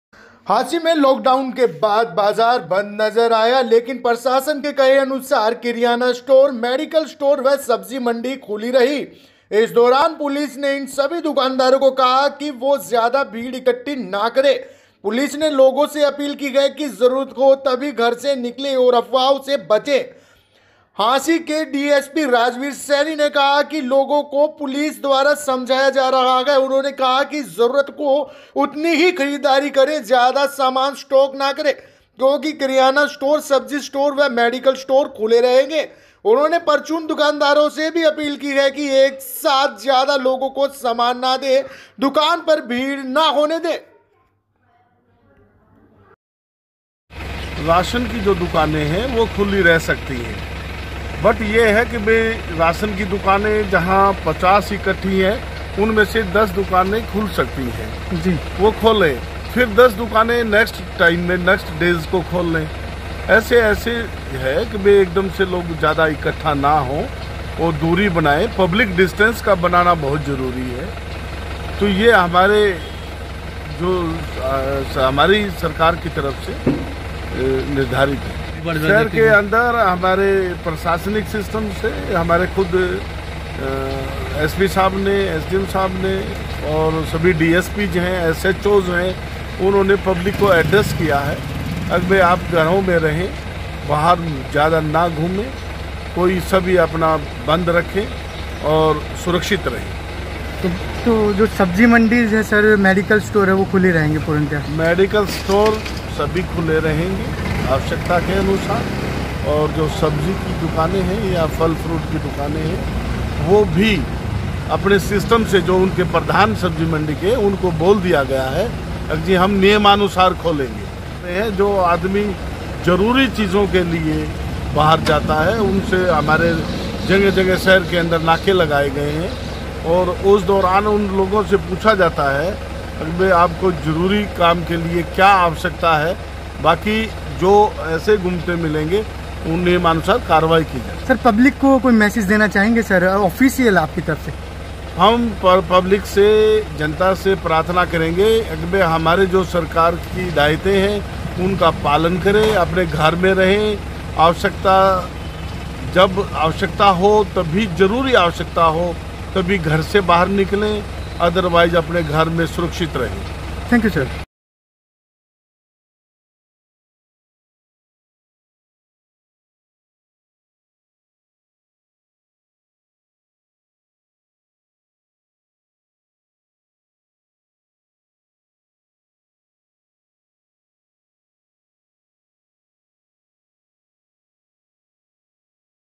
लोक डाउन के बाद बाजार रहे बंद, सब्जी मंडी,किरयाना स्टोर मेडिकल स्टोर रहेंगे खुले
"ANT national"ख़बर वही जो हो सही,हांसी,लोक डाउन के बाद बाजार रहे बंद, सब्जी मंडी,किरयाना स्टोर मेडिकल स्टोर रहेंगे खुले